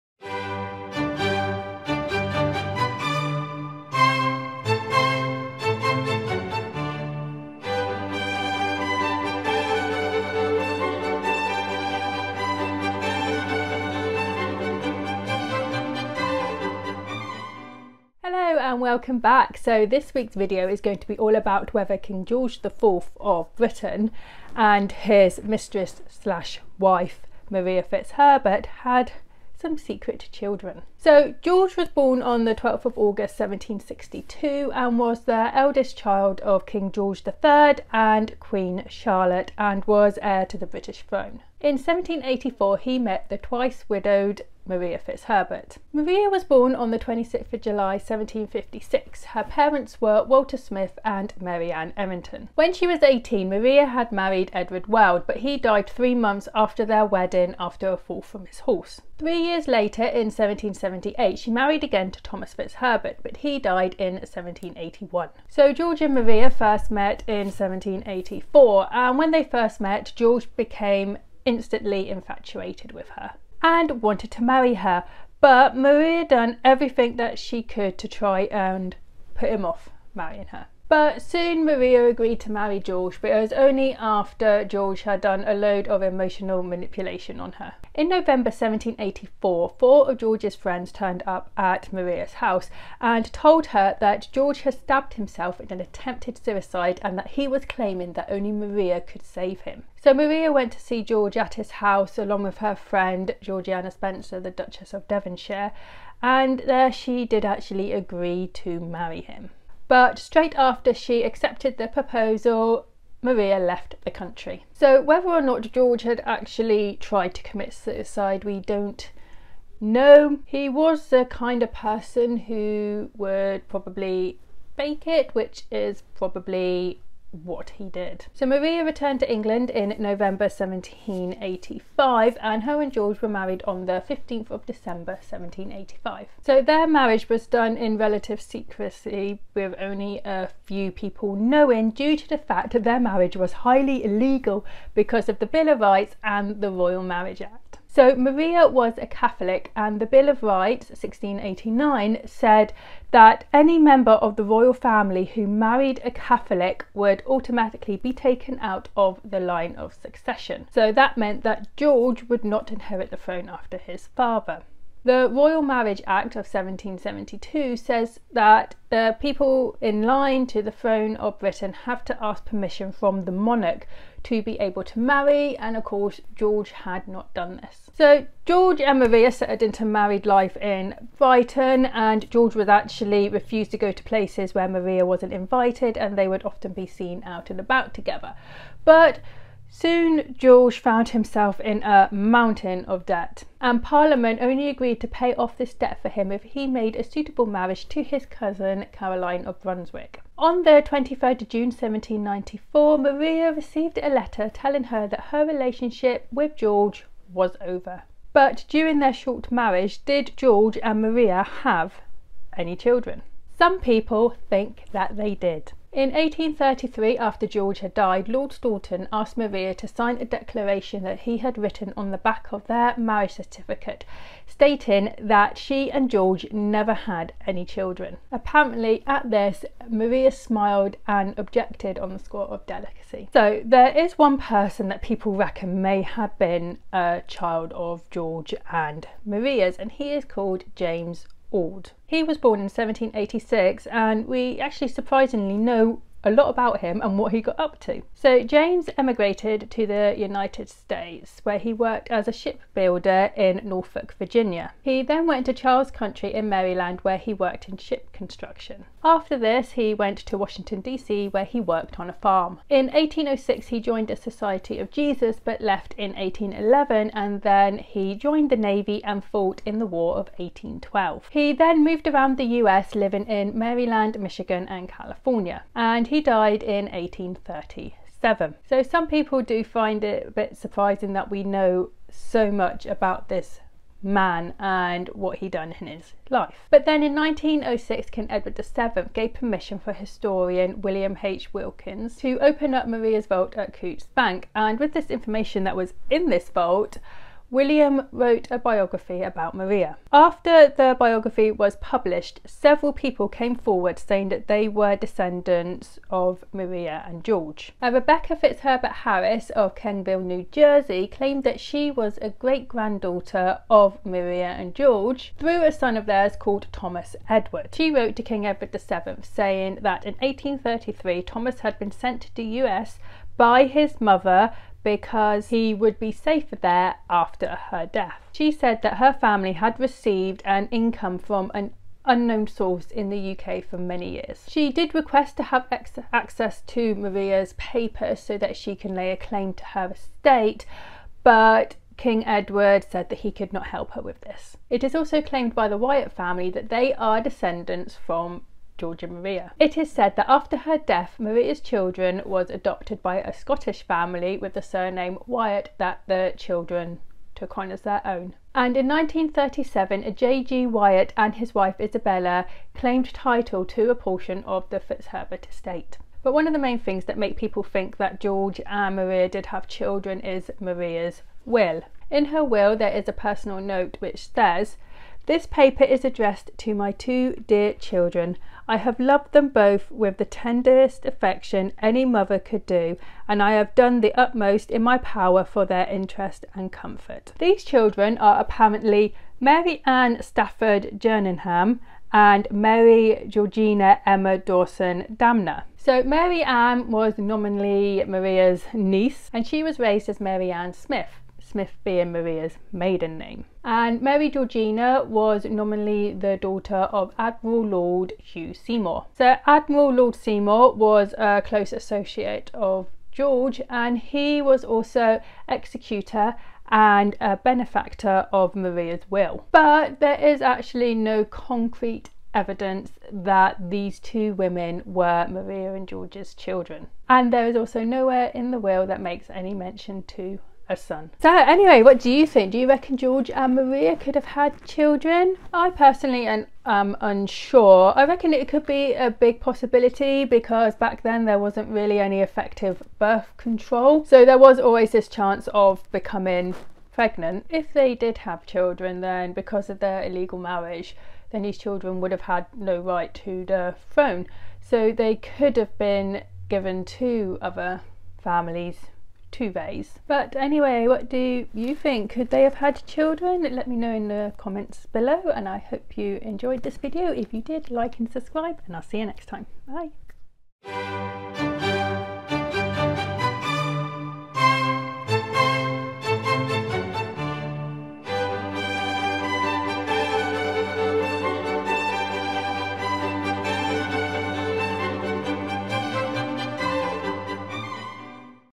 Did King George IV and Maria Fitzherbert have children?
in 1785 King George iv of Britain made an illegal marriage to his mistress Maria Fitzherbert. he soon left her after parliament said that they would pay his debts if he married his cousin Caroline. but during their short marriage did George and Maria have any children?